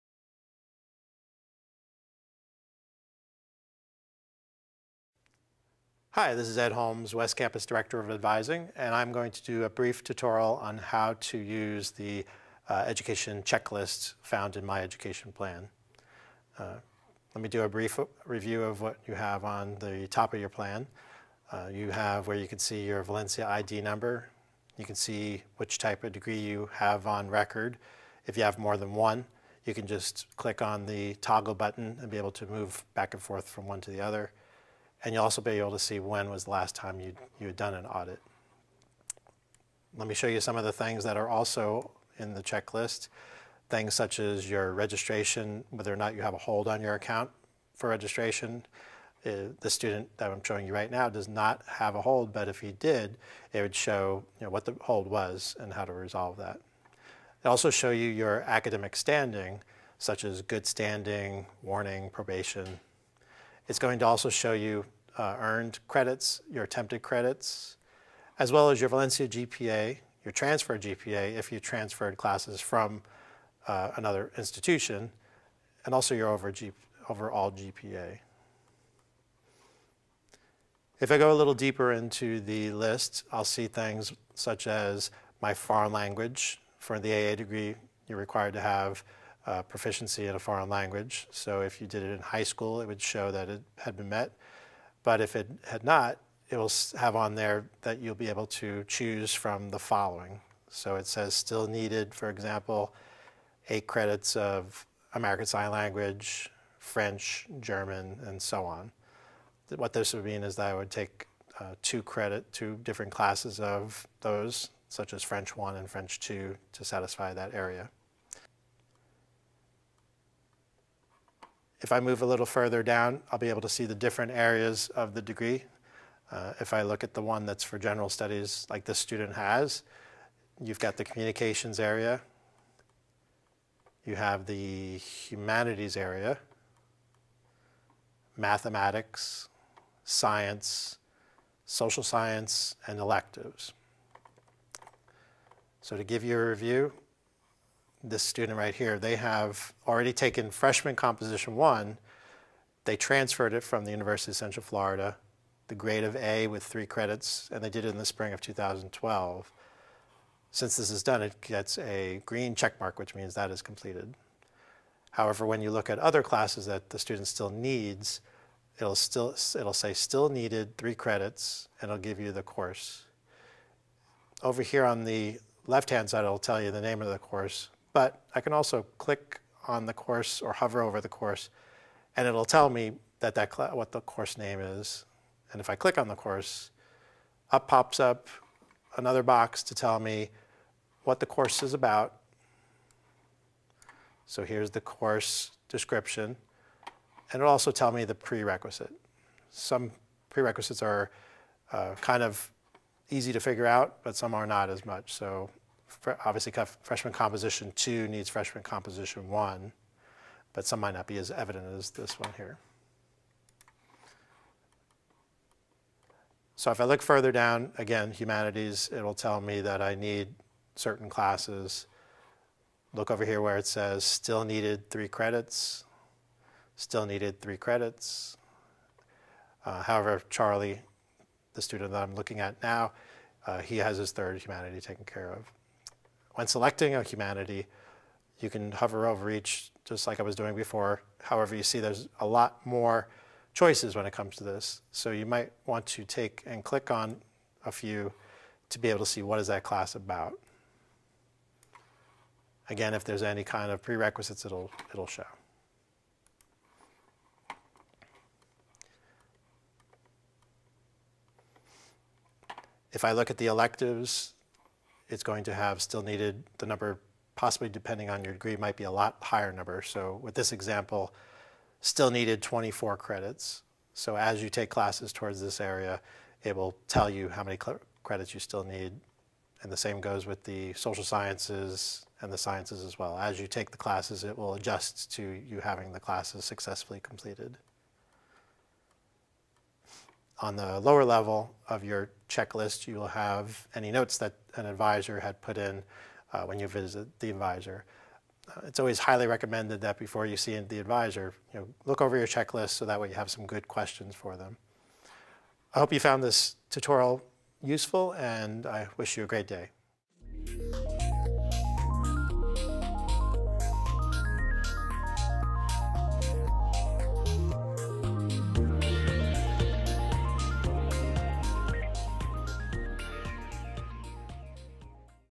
MEP "Checklist"
This tutorial is about how to use the "Check List" feature within My Education Plan (MEP)